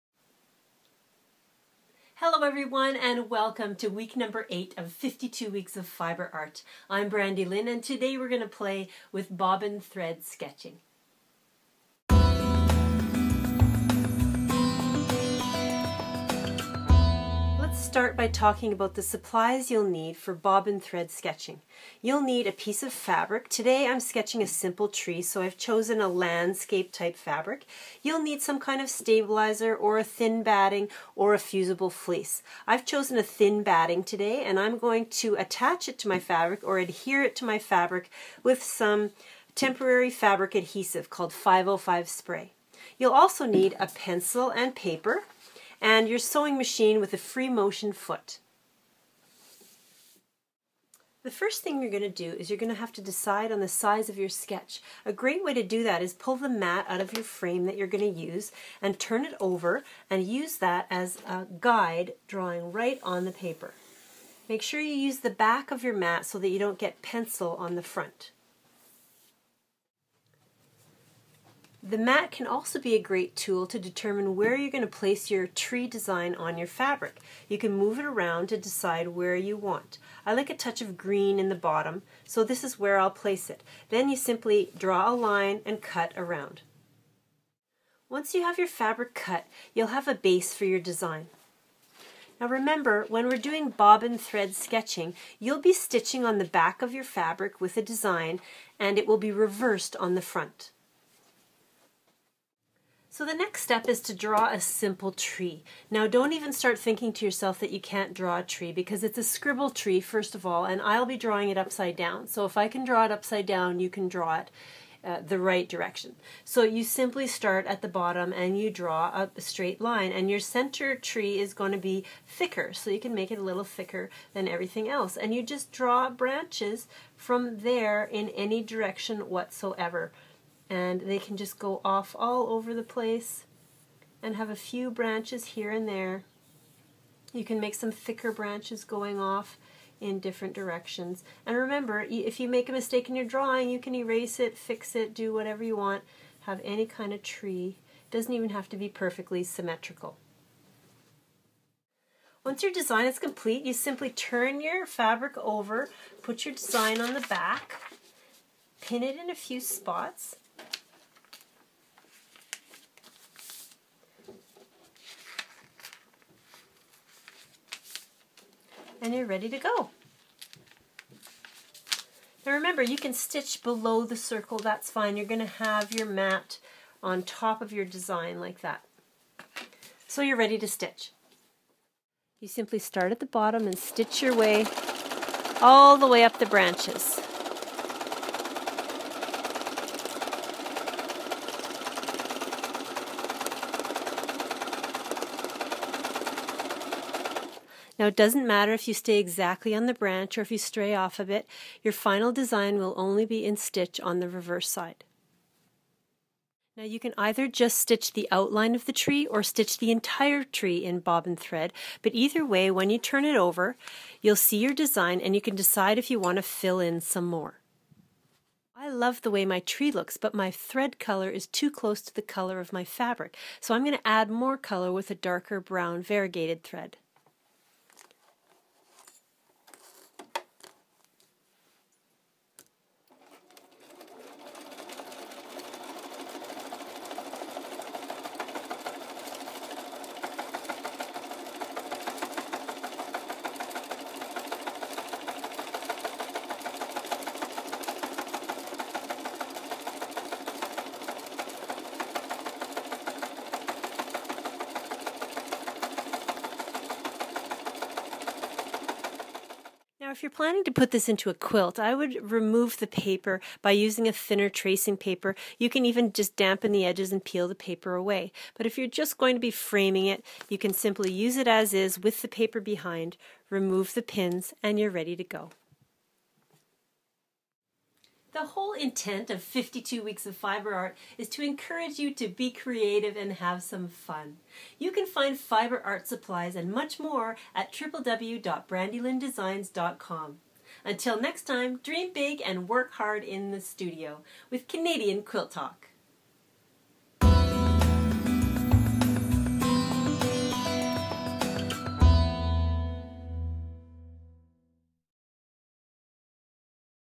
Week 8 Bobbin Sketching
Transfer your sketches to fabric fast and easy with bobbin thread sketching. So fun and simple you'll be sketching the night away.